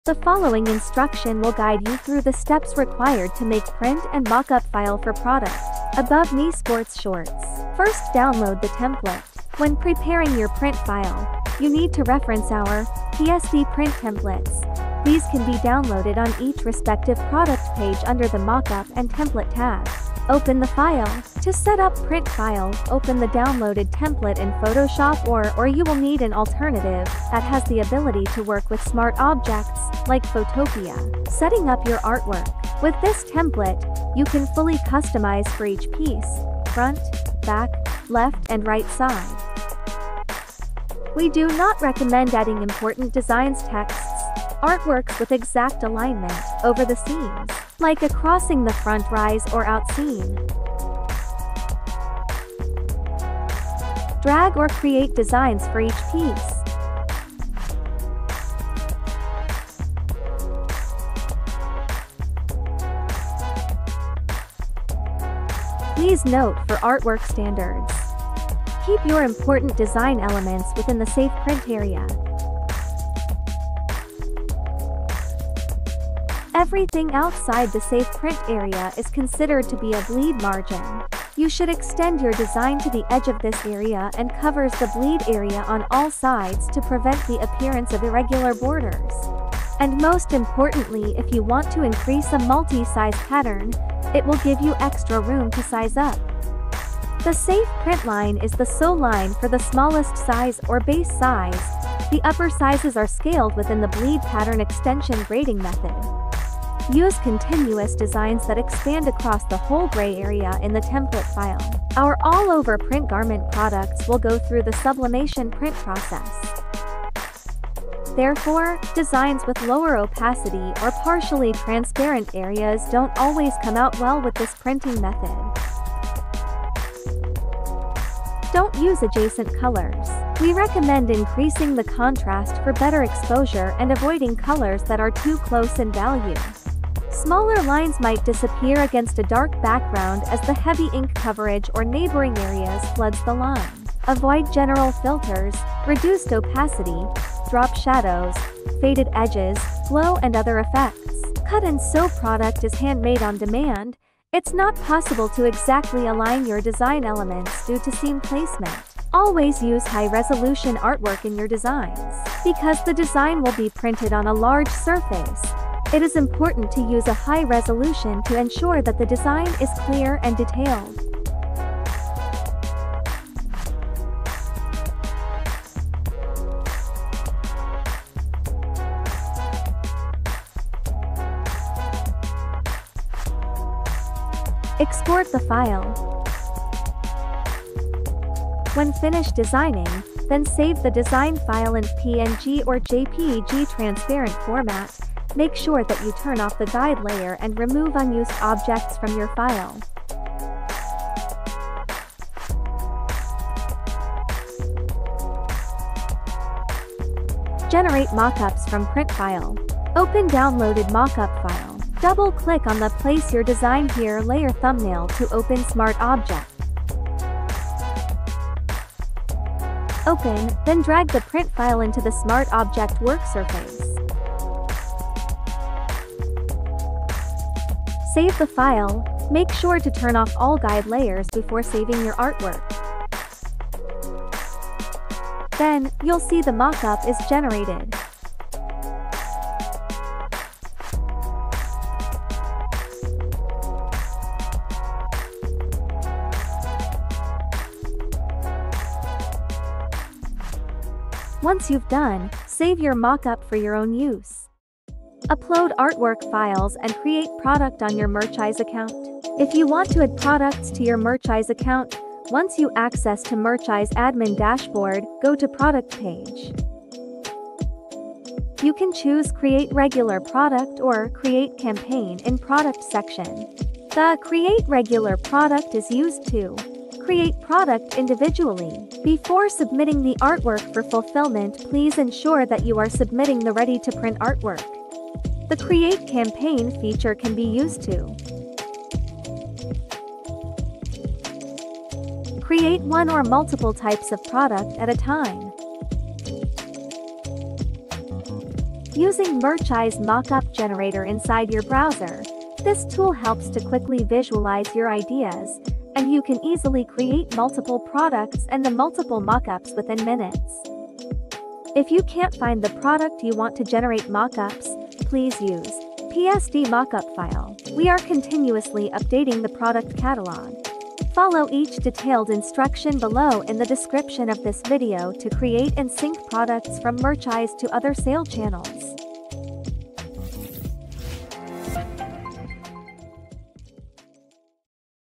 💢 Featured Products ⚡ Sports Shorts I Template & Mockup Guides
In this video, we’ll walk you through the steps required to create print files for embroidered sweatshirts using our Above-knee Sports Shorts product. We’ll cover everything from downloading the template to placing your design to submitting the artwork for fulfillment.

*** Follow each detailed instruction below in the description of this video to create & sync products from Merchize to other sale channels.

*** Follow this guide to create manual orders in Merchize